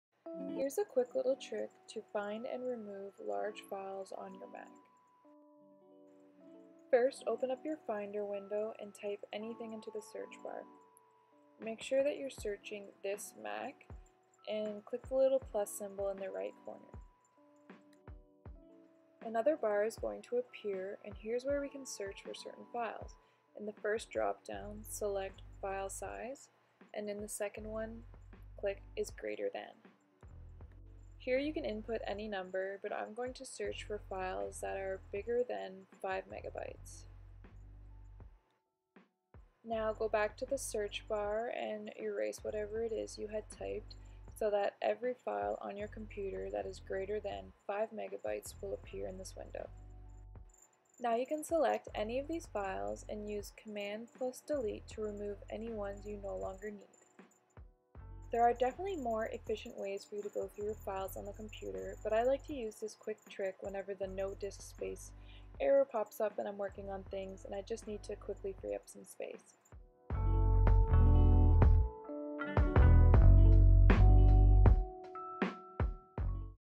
Find and Remove Large Files from your Mac
There’s nothing worse than receiving the dreaded ‘your disk is almost full’ notice when working on your Mac. As a designer, I get this notice more than I care to admit — I’m constantly downloading large design asset files, and admittedly have terrible file management skills.

Luckily, there is an easy way to find large individual files on your Mac, and delete the ones you don’t need.

You can do this using the Finder. Here's how!